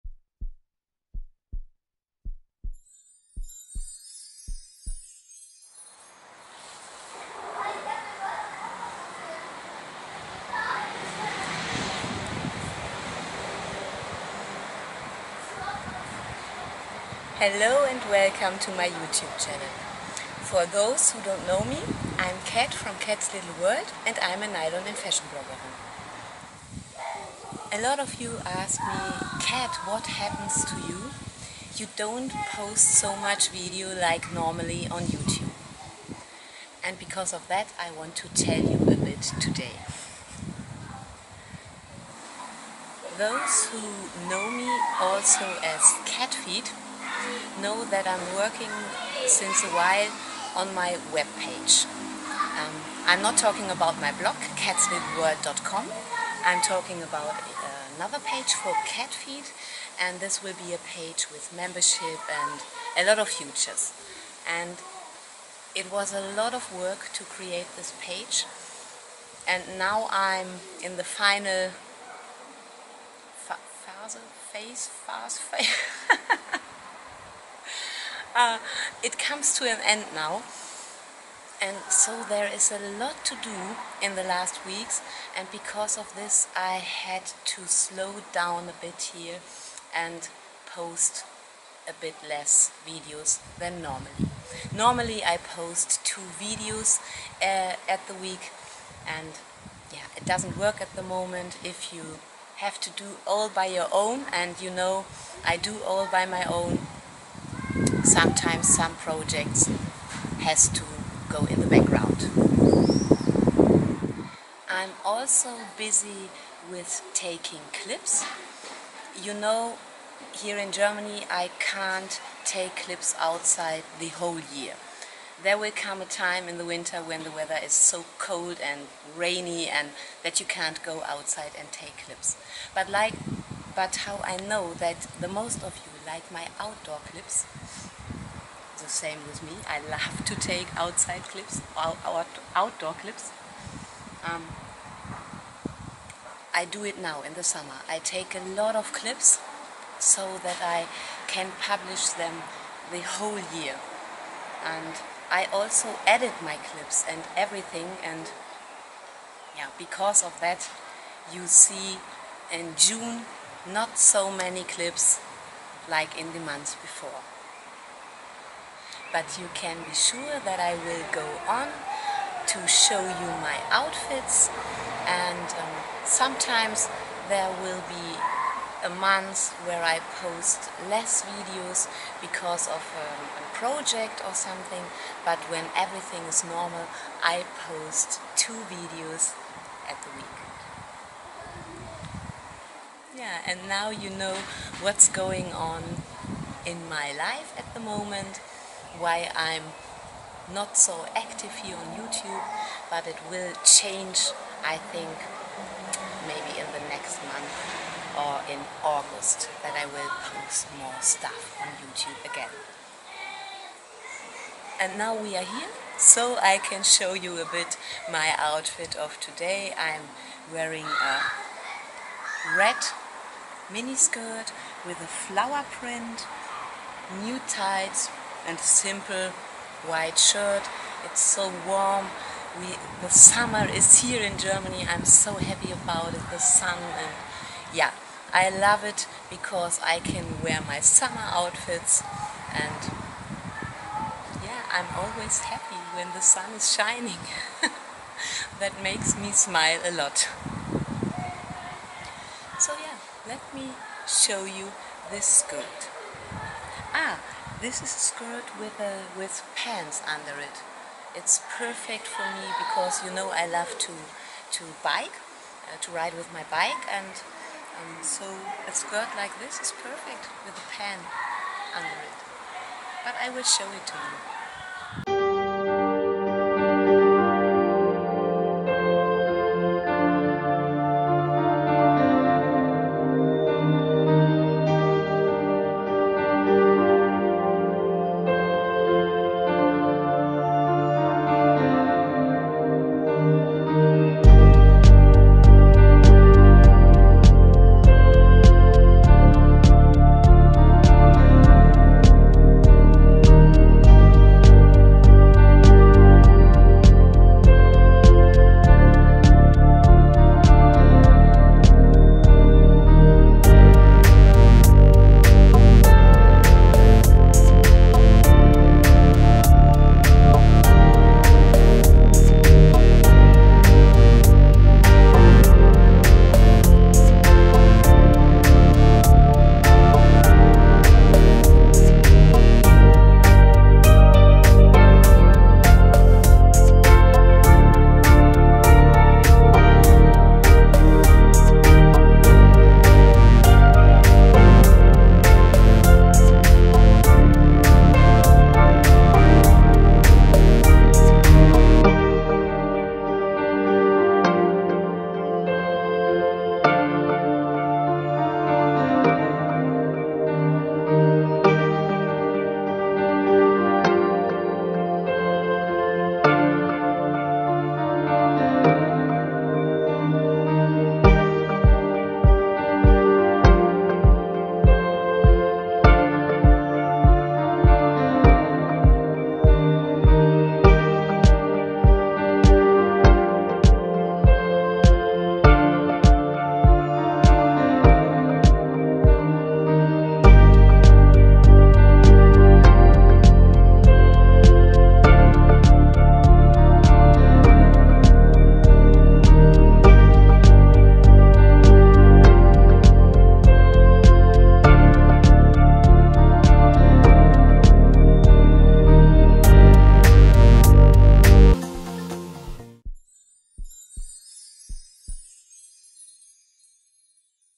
MINI SKIRT and TIGHTS - News about Kats little world
Hello,
today I show you my red mini skirt with floral print and nude tights while I tell you why it was too quiet on my channel and what is going on in Kats little world ;)
Love Kat